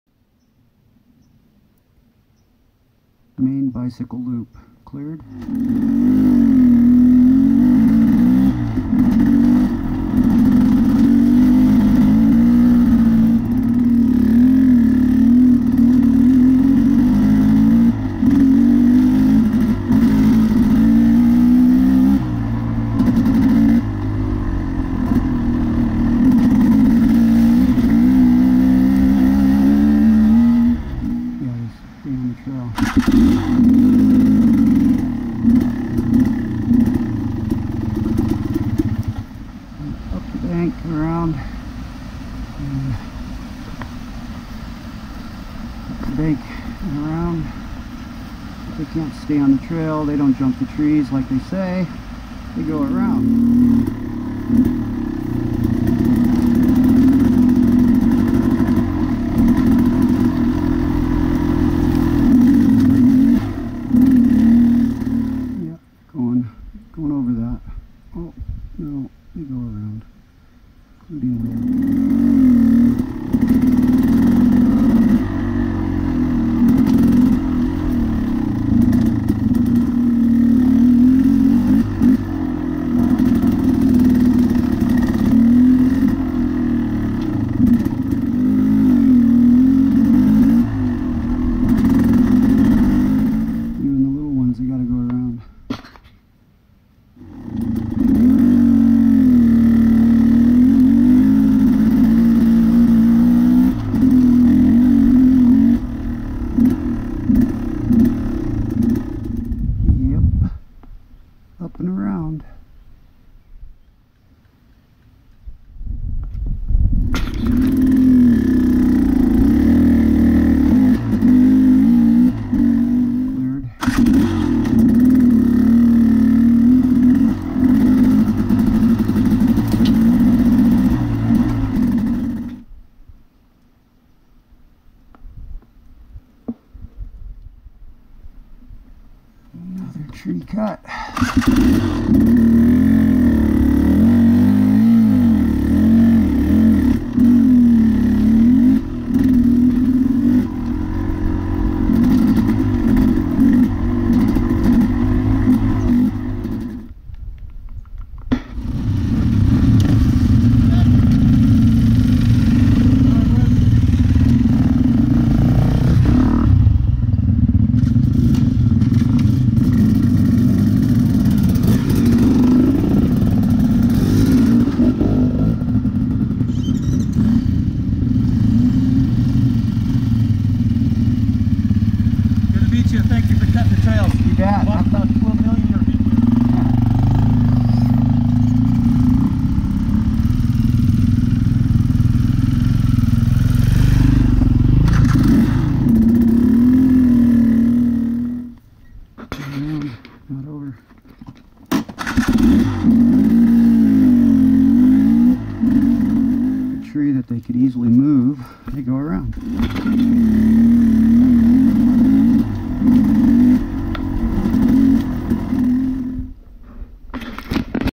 One Rider and a Chainsaw
Another trail through a burn area, cleared.  With all the dead/burned trees, more trees will fall.  This is why you need a chainsaw in your group.  Trees are falling, daily.